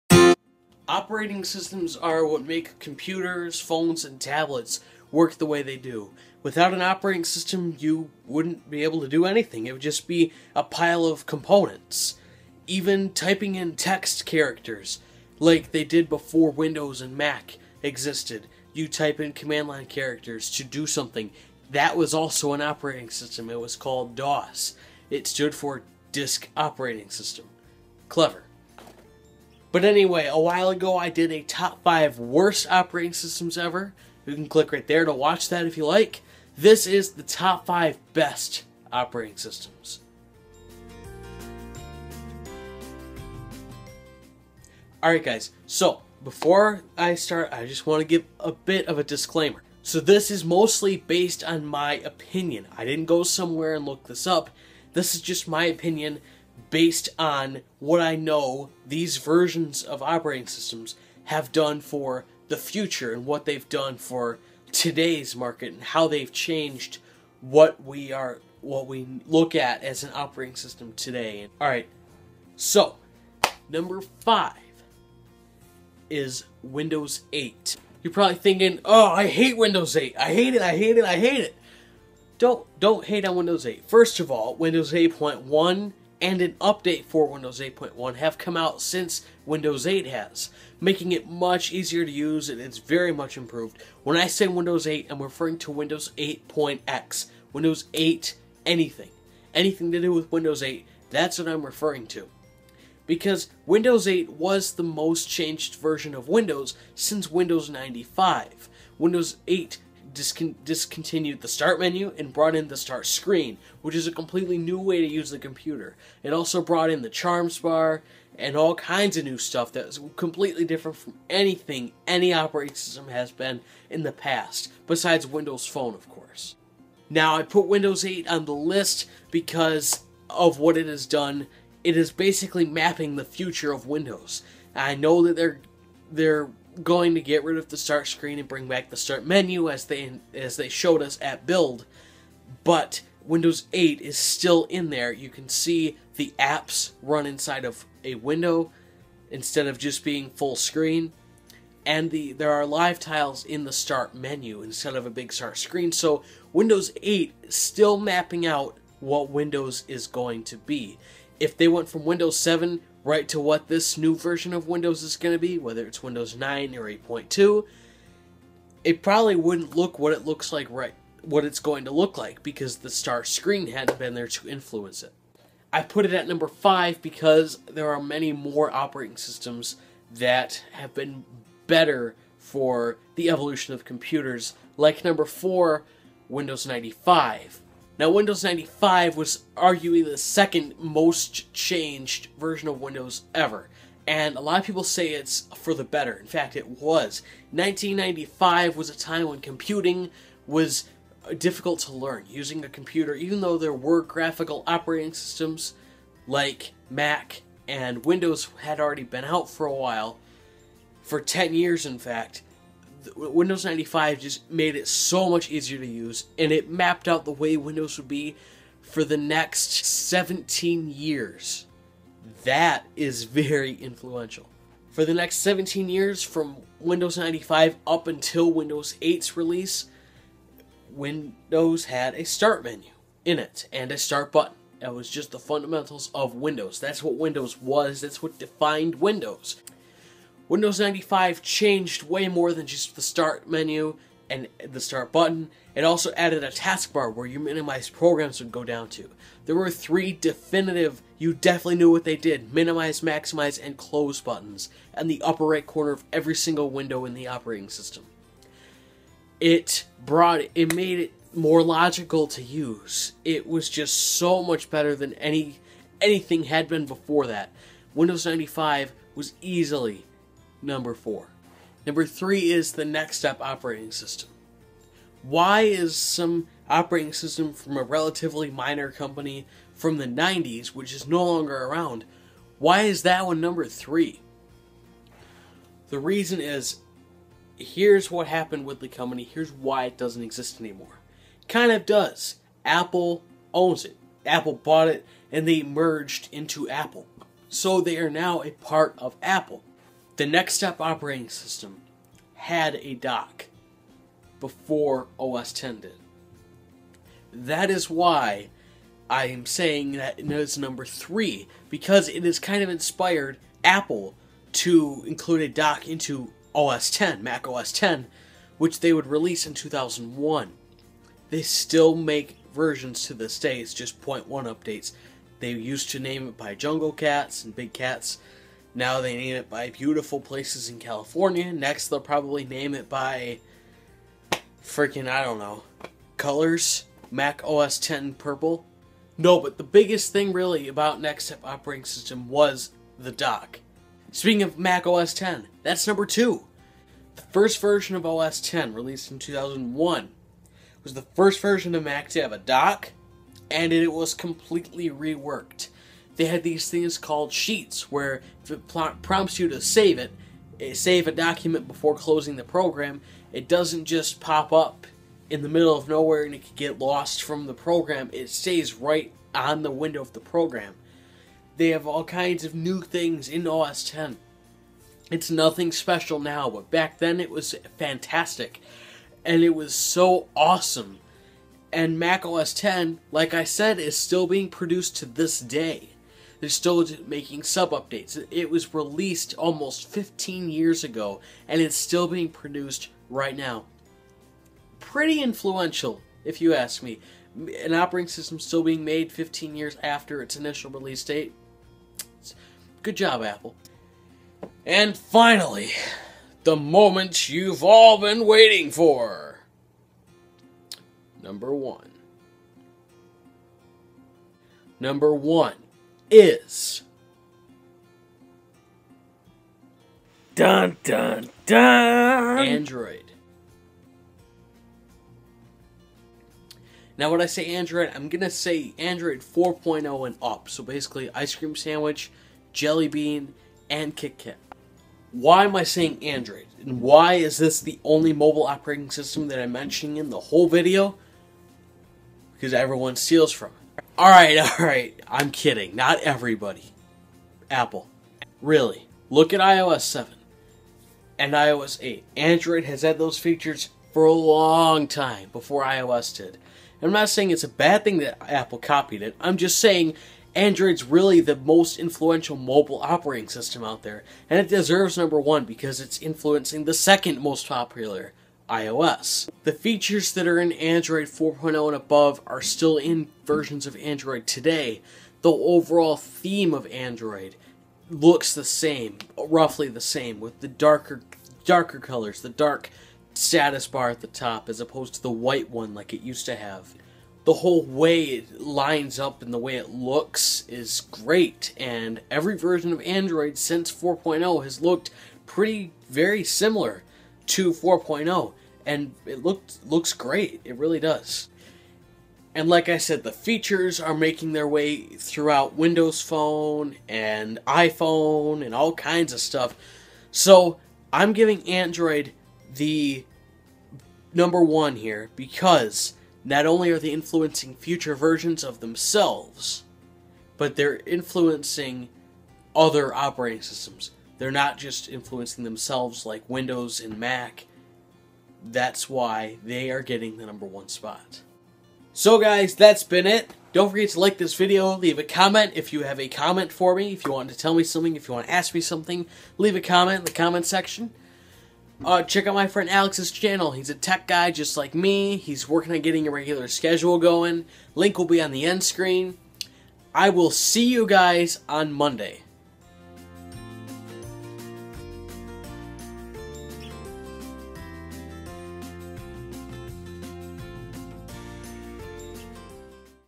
Top 5 Operating Systems (Most Influential OS's)
Operating systems are fundamental for the use of any piece of technology. There are countless operating systems in the world, but what are 5 of the best?